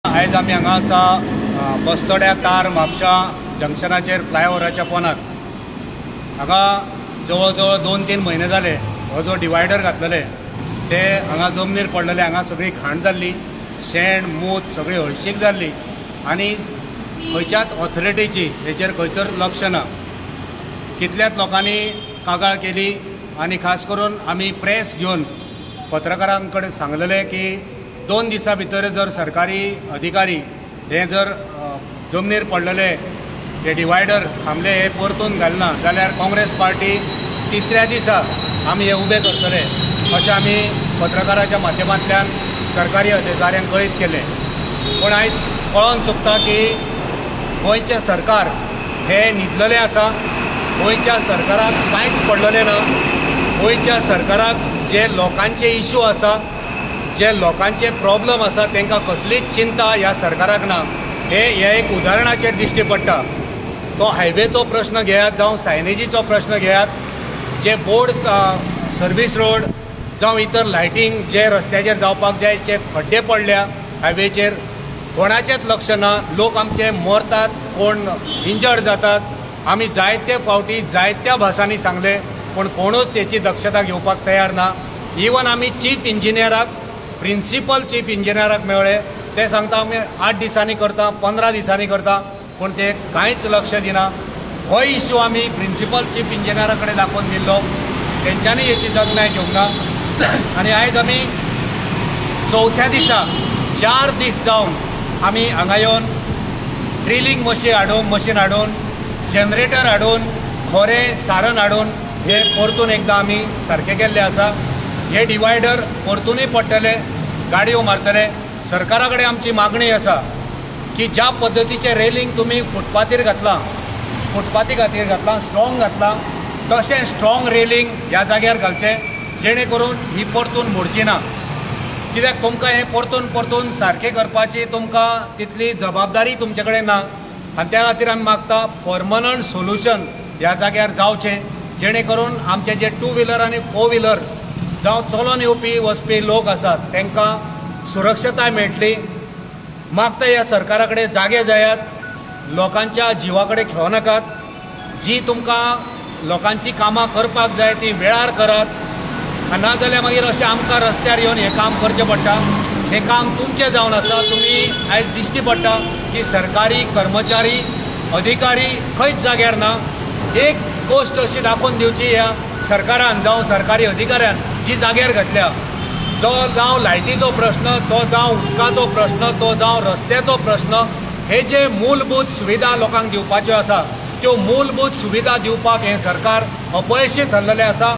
CONGRESS FIXED DIVIDERS OF UNDER PASS [FLYOVER] & CLEANED THE AREA AT MAPUSA -TAR-BASTORA
RDXGOA NEWS CHANNEL  | Most Popular Goa News Channel